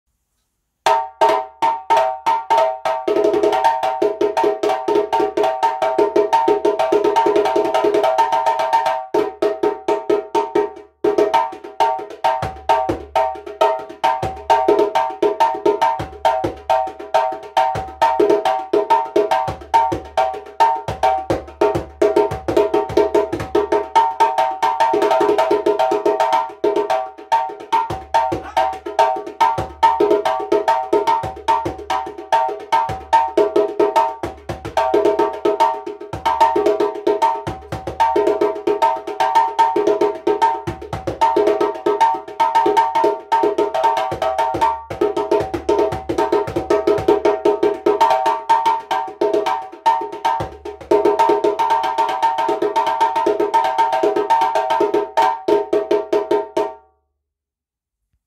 Djembe repair - overhaul with a calf skin - sound demo Moussa Coulibaly
Repaired calf skin djembe played by Moussa Coulibaly.